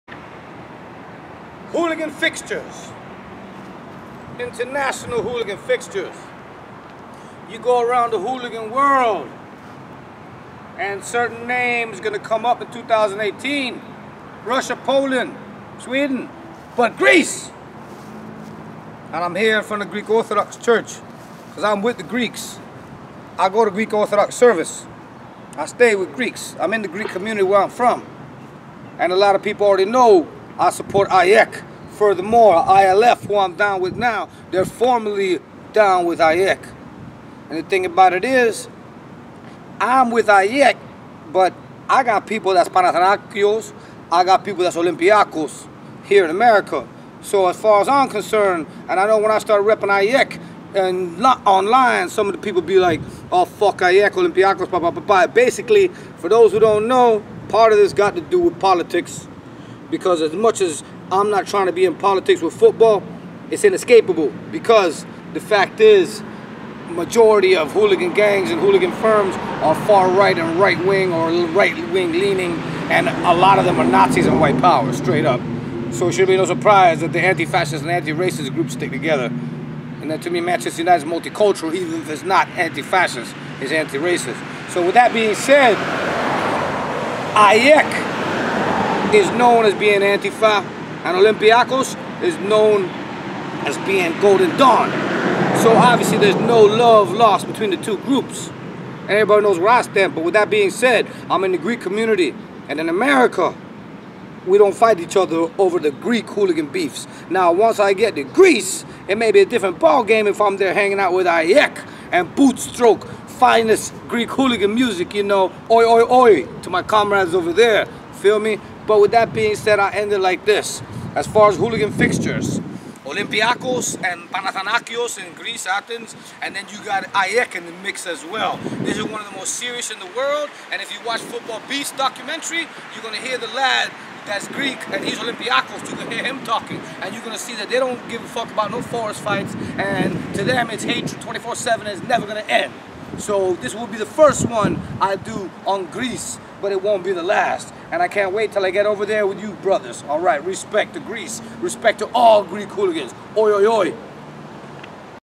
Athens, Greece Hooligans: Top Fixtures 2
The exciting world of Football Hooliganism is chock full of compelling conflicts. As this Street-level Culture has spread around the world, it has taken shape in a myriad of forms colored by the local culture and Football passions it manifests from.

In this second installment covering international Hooligan fixtures or intense rivalries, U.S. Hooliganism Expert DIABLO discusses one of the most volatile Hooligan scenes in the world; ATHENS, GREECE.

There is and has been a Street War going on in Athens between Supporters of Football Clubs AEK (black and gold), Panathanakios (green and white), and Olympiakos (red and white) that has only intensified over time.